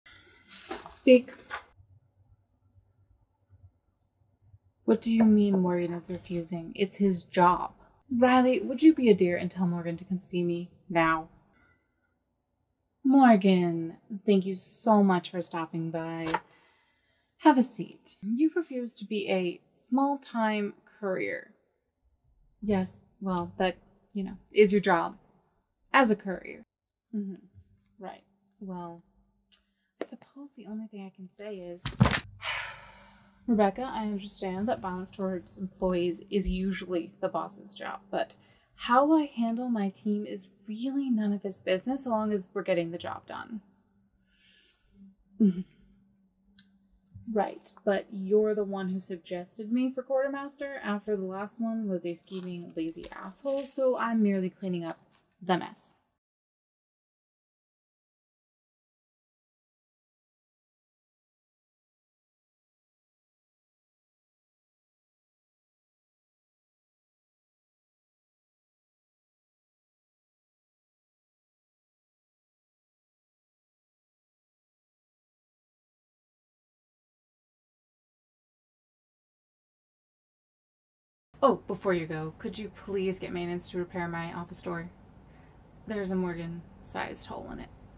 Hole in the Door | Ash Westleye Ep 2
Another door to replace? Ash Westleye just wanted a moment of peace... Inspired by the world of Hannah Nicole Mae